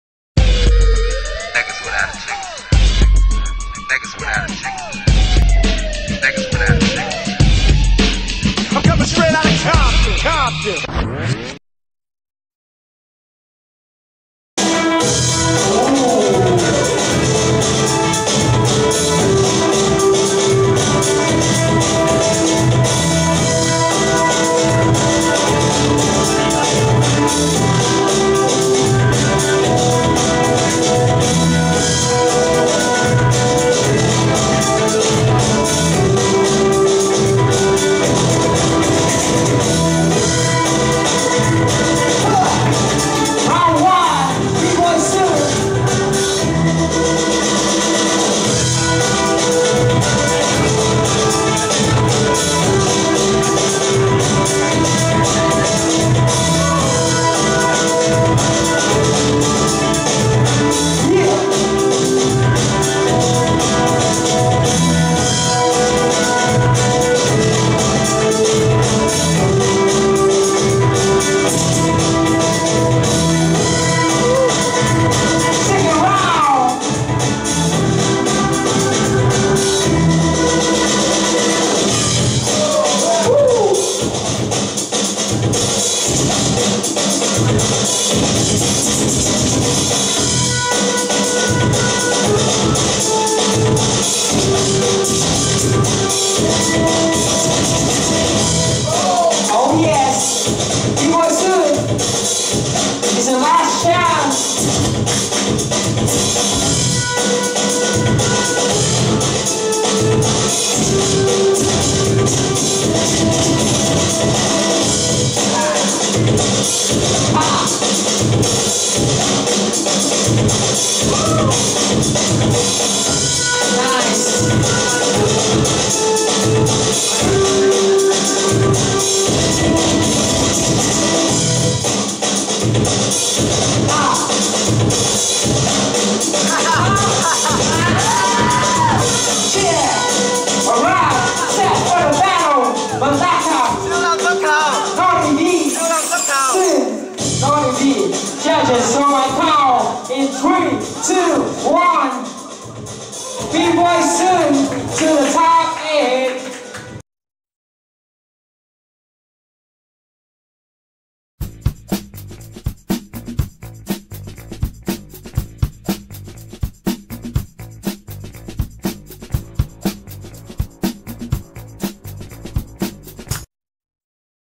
DARKNESS WAR 2013 |BBOY 1ON1 | TOP16 |BBOY SOON VS BBOY NAUGHTY B | BBOYWORLDMALAYSIATV
WHO YOU THINK WIN THE BATTLE.? SHARE YOUR COMMENT

VIDEOGRAPHER - AHSOON(BBOYWORLDMALAYSIA TEAM)

NOCOPYRIGHT